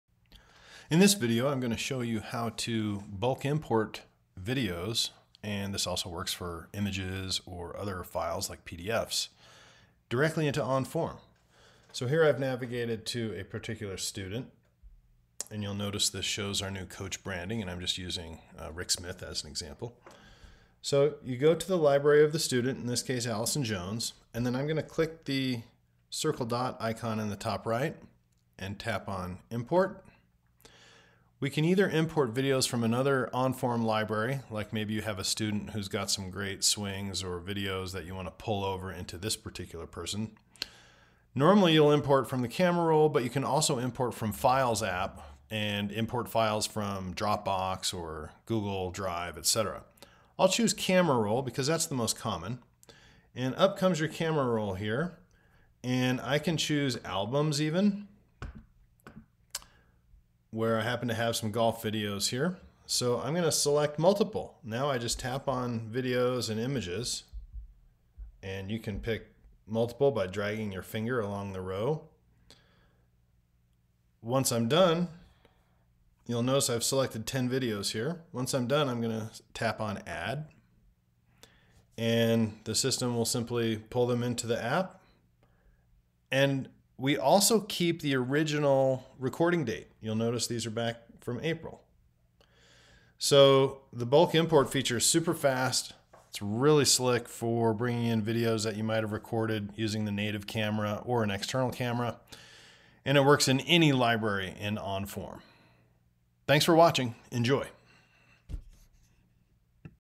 Bulk Import Multiple Videos and Images
NEW! With our latest release, version 2.01.6 you can easily import multiple videos at once. This process works for videos, images, PDF's and other documents. You can import from the camera roll on your device or from Dropbox, Google drive etc. through the Files app.  This is an iOS only feature.